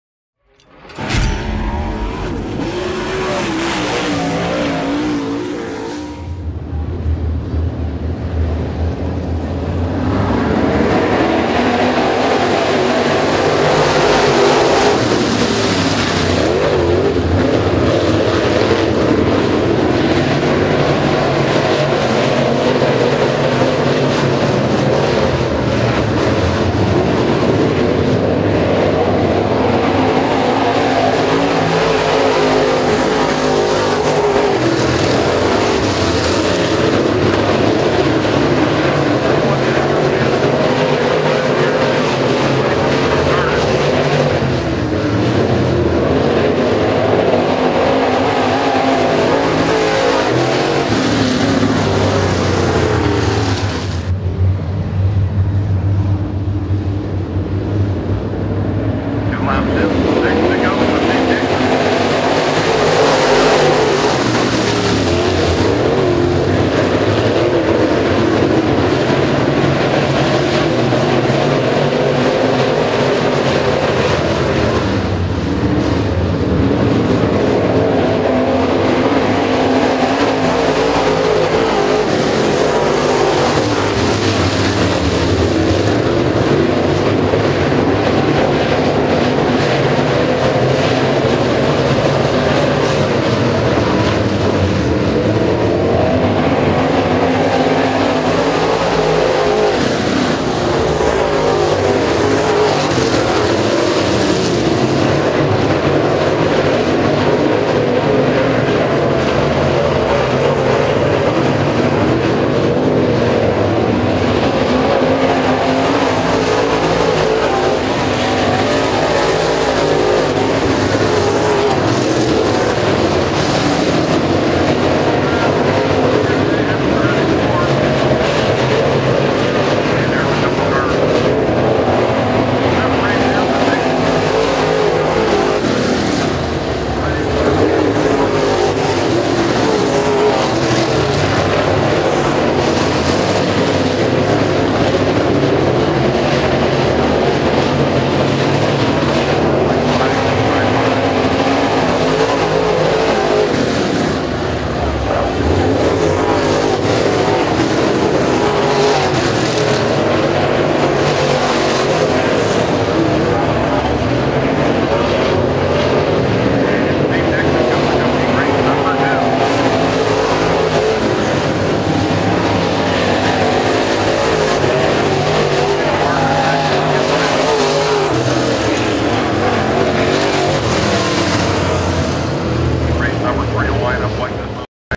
RUSH Crate Late Model Heat Two | Stateline Speedway | 6-5-21
RUSH Crate Late Model Heat Two at Stateline Speedway (Busti, NY) on Saturday, June 5th, 2021!